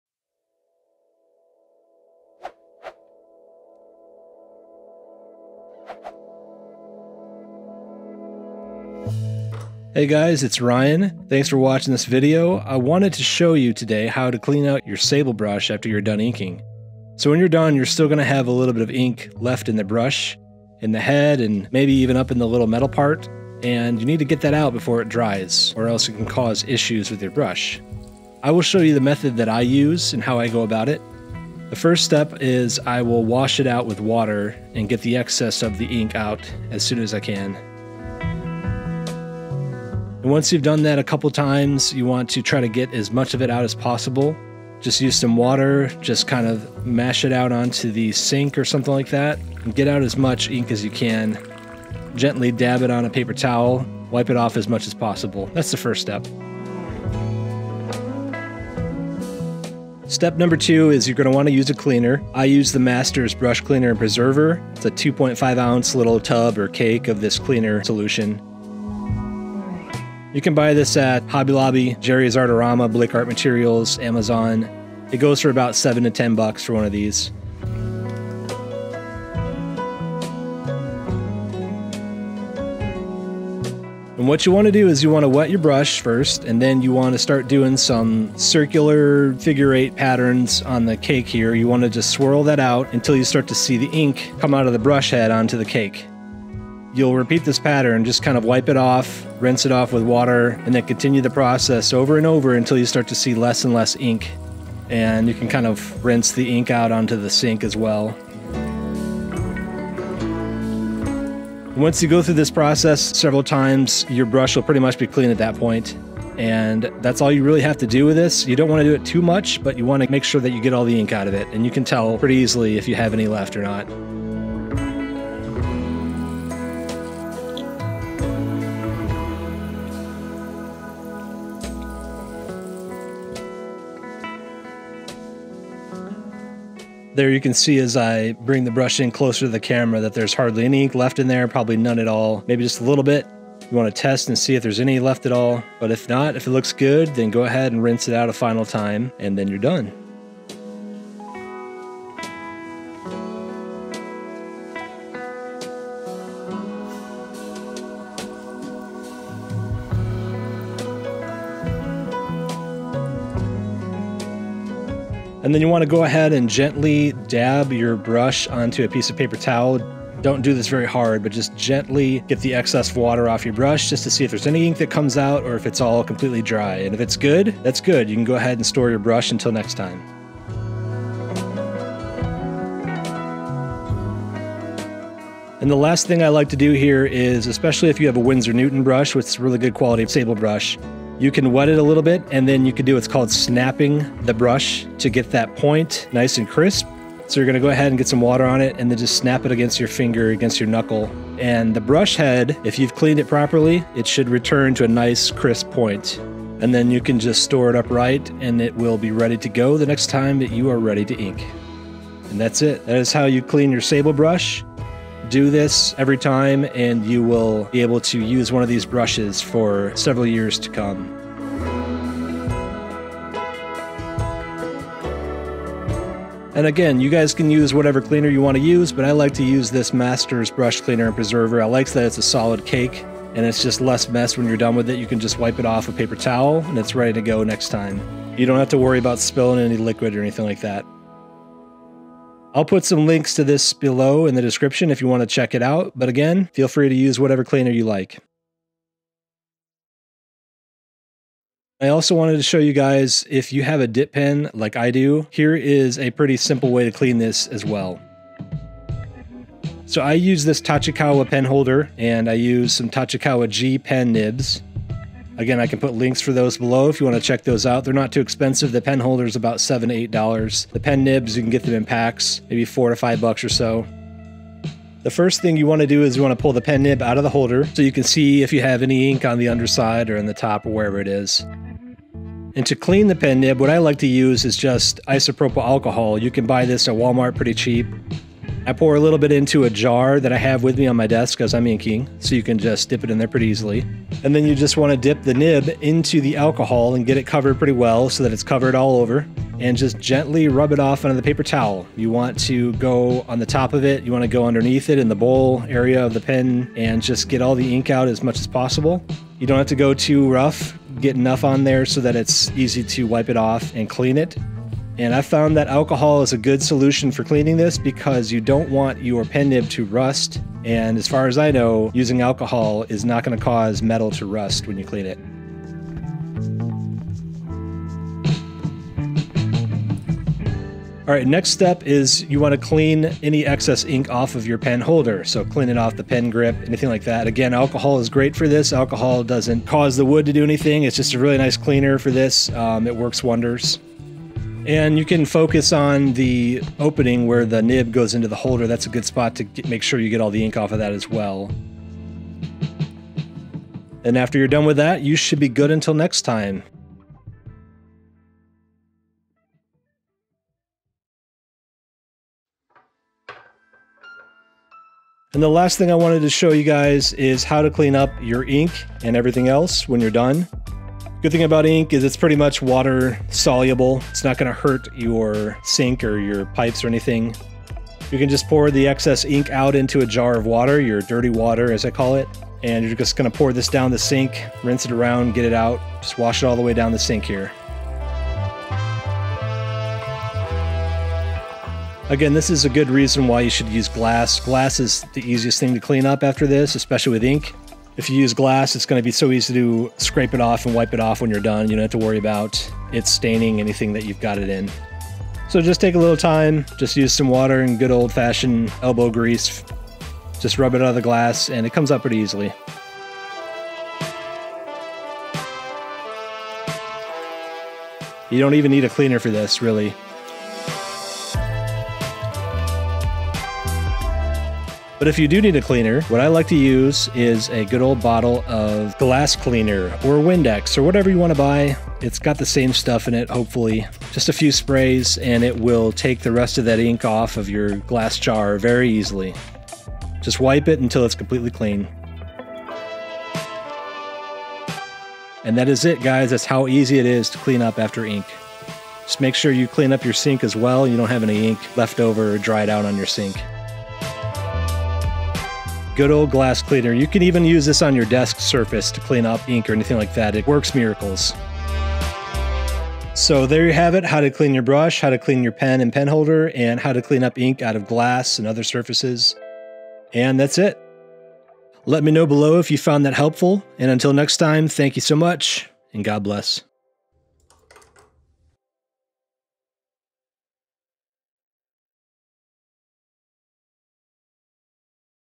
How to Clean a Brush (or Pen) After Inking or Painting
Step by step guide showing you how I usually clean up after inking with a sable brush and/or with a pen. I show you the brush cleaner that I use to remove remaining ink, and how I clean my pen nib so no ink remains or anything causes it to rust.

***The Masters Brush Cleaner & Preserver***

***Winsor & Newton Series 7 Sable Brush (various sizes)***

***Tachikawa T-40 Pen Holder***

***Zebra G Pen Nibs***

0:00 Intro
0:10 Cleaning a Sable Brush
4:09 Cleaning a Dip Pen
5:59 Cleaning Up Ink